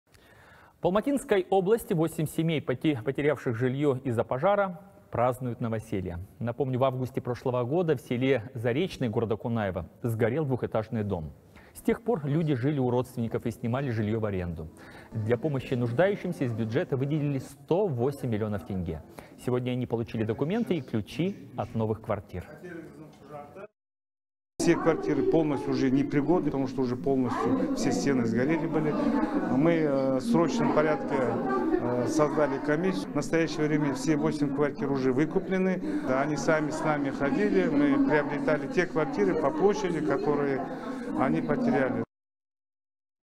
Ключи от новых квартир вручили погорельцам из Заречного
В Алматинской области 8 семей, потерявших жильё из-за пожара, празднуют новоселье, сообщает «24KZ».